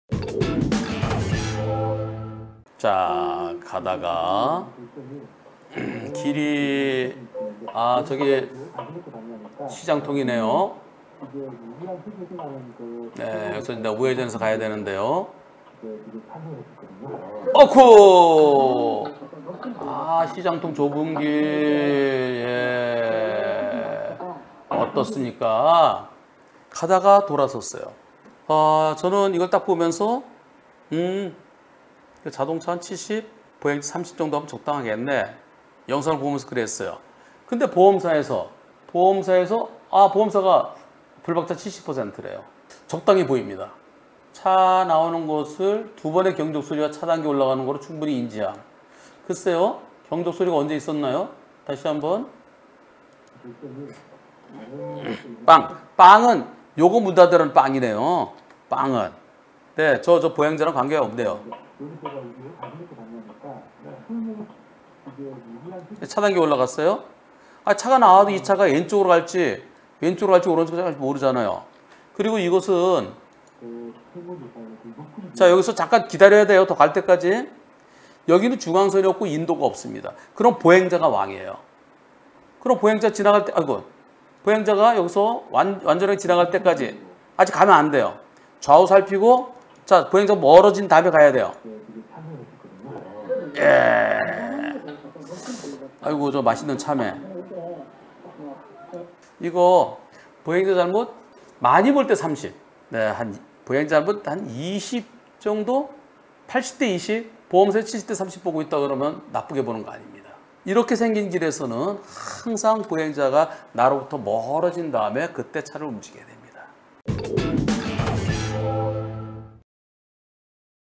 22524회. 이런 도로에선 사람을 멀리해야 좋습니다
70092
240713 토 생방송
좁은 골목길에서 우회전 출차하다가 가던 보행자가 멈춰서면서 사고

영상을 보자마자 70대30 을 생각함,
보험사에서 70대30이라고 함,
보행자 잘못 20~30% 정도 인데 보험사에서 70대30 보고있으면 나쁘지않음,
이런 곳에선 사람이 멀어진 다음에 움직이는 것이 안전,